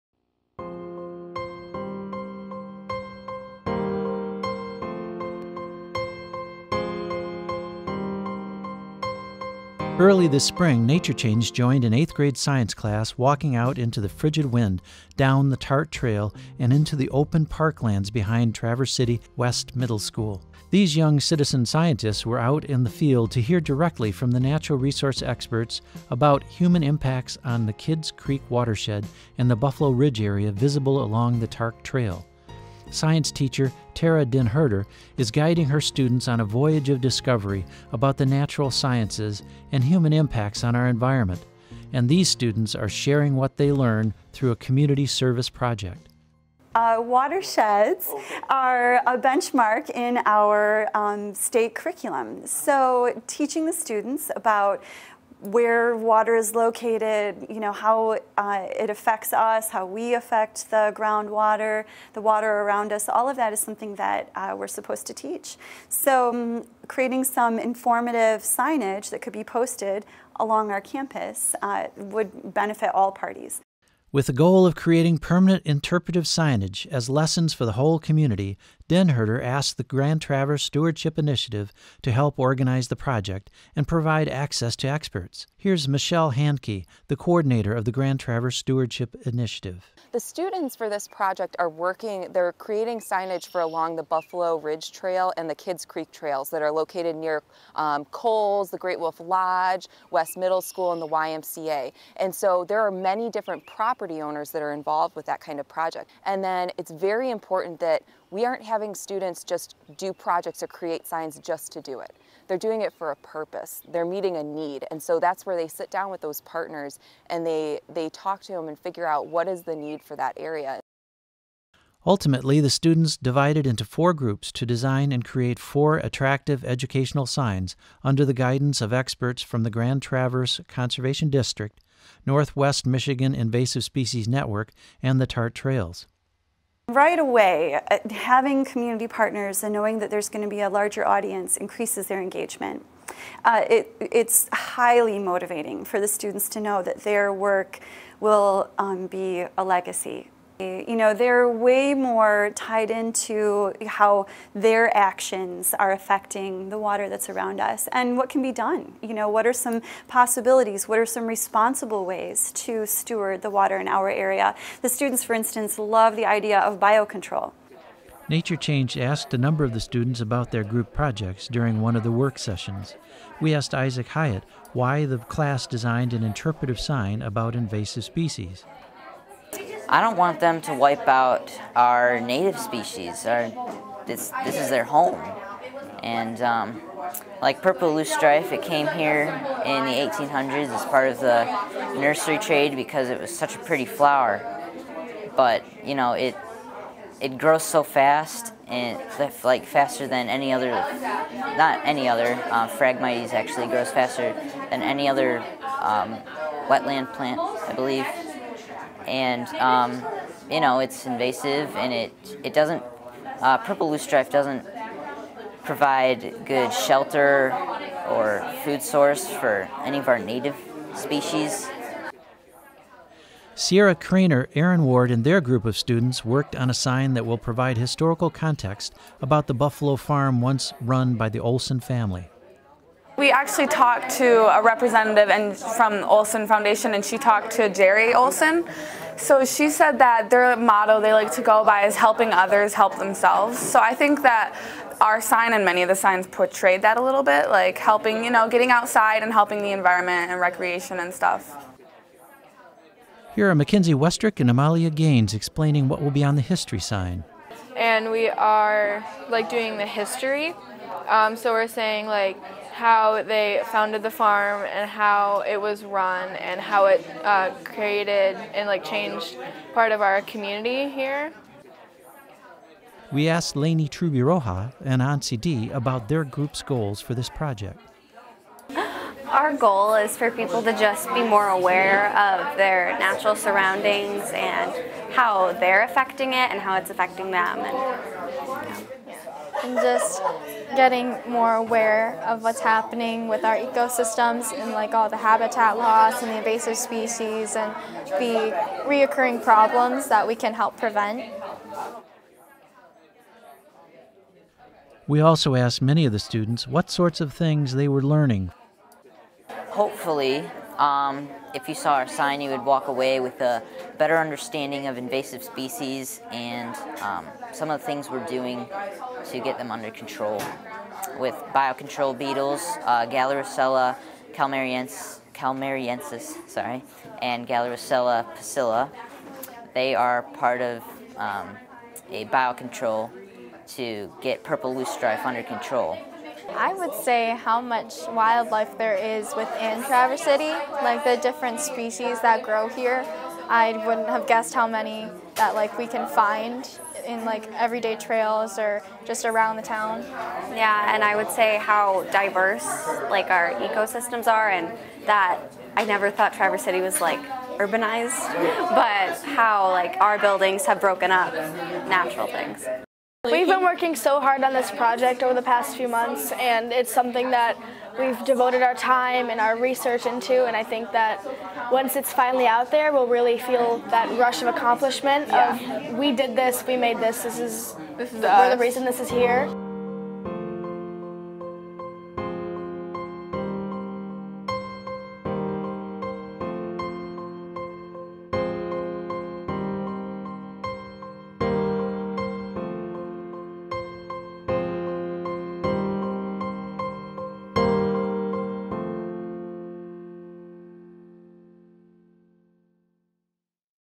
Creating Interpretive Signage for the Kids Creek Watershed
In this video, we hear from some very bright and dedicated young citizen scientists working to share what they have learned about the Kids Creek Watershed and the Buffalo Ridge Trail in Garfield Township, near Traverse City.

Early this past spring, Nature Change joined an 8th grade science class walking out into the frigid wind, down the TART Trail and into the open park lands behind Traverse City West Middle School.
Huddling together at different points along the new trail, these young citizens scientists were out in the field to hear directly from natural resource experts about human impacts on the Kids Creek Watershed and the Buffalo Ridge area visible along the Tart Trail.

Science teacher, Tara DenHerder guided her students on a voyage of discovery about the natural sciences and human impacts on our environment. And these students are sharing what they learned through a community service project - interpretive signage they created and helped install on site.